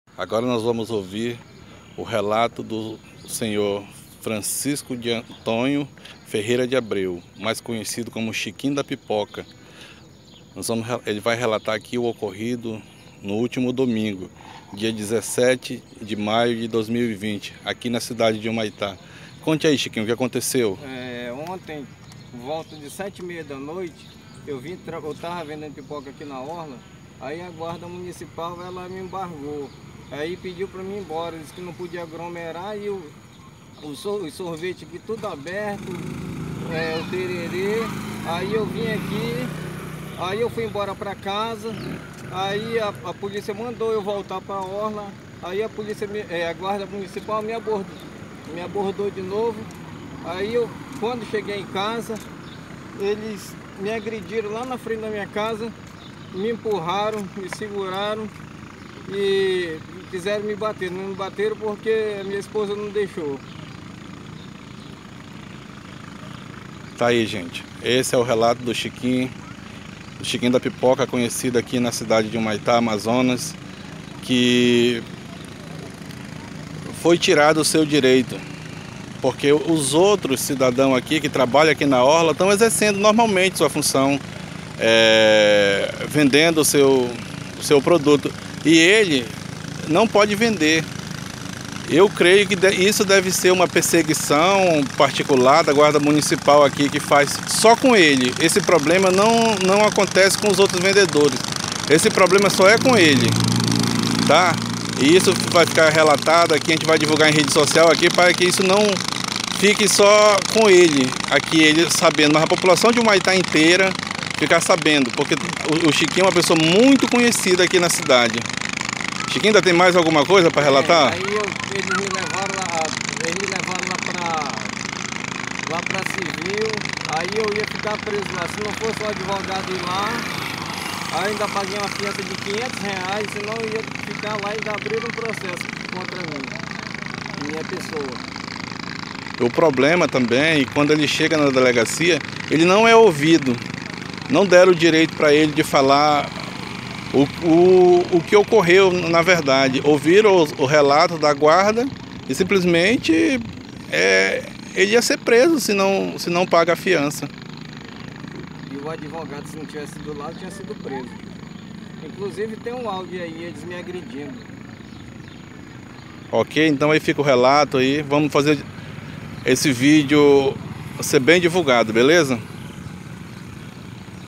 O Chiquinho da Pipoca fala sobre a coação que sofreu para não trabalhar em Humaitá- Am
Foi levado inclusive para delegacia e se não fosse o advogado seria preso, segundo este vídeo que esta rolando nos grupos.